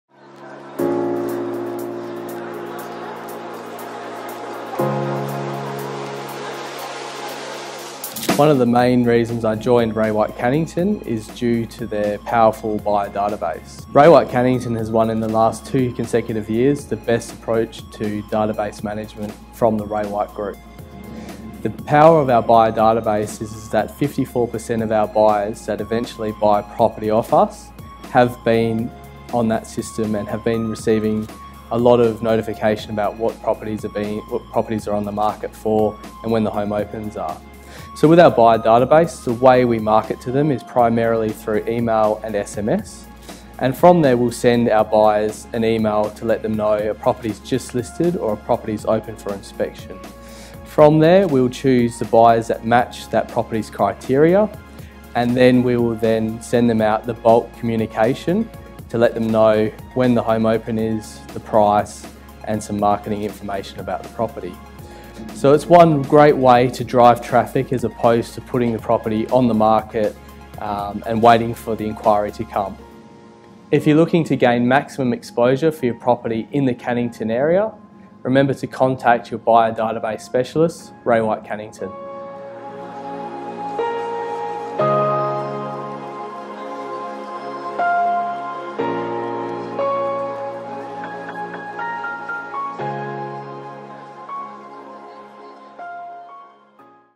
Power of Ray White Canningtons Buyer Database
Charlie Bellow gives quick introduction into the power of Ray White Canningtons buyer database